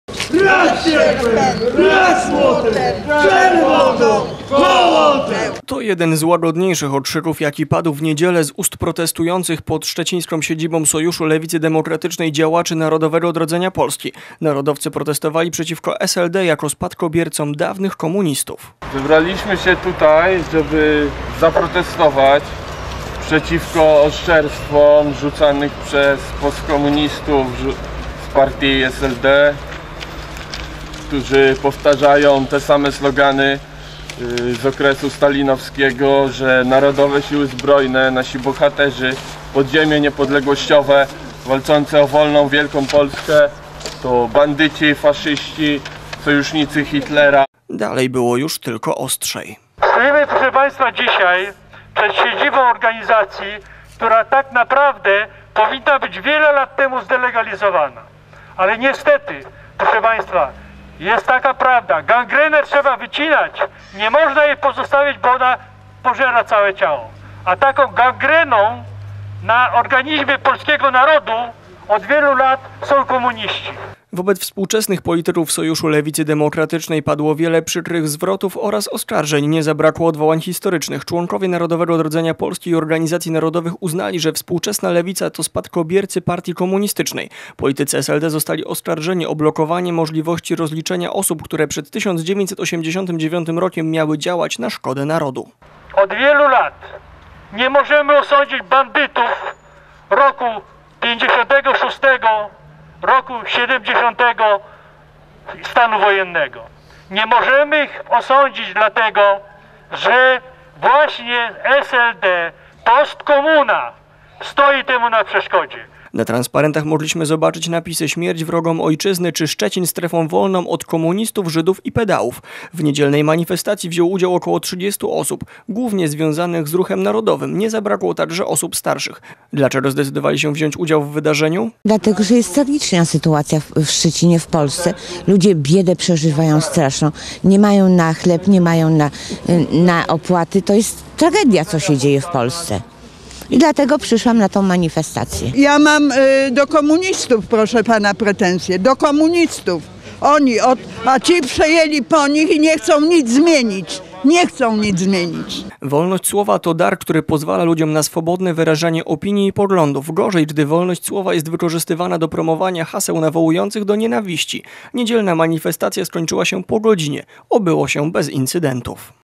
NOP chce delegalizacji SLD
Śmierć wrogom ojczyzny to tylko jedno z haseł jakie na transparentach mieli przedstawiciele środowisk narodowych domagających się w niedziele delegalizacji Sojuszu Lewicy Demokratycznej. Dlaczego? Bo to spadkobiercy komunistów. Co jeszcze nie podoba się narodowcom w działaczach lewicy? Odpowiedź w materiale filmowym.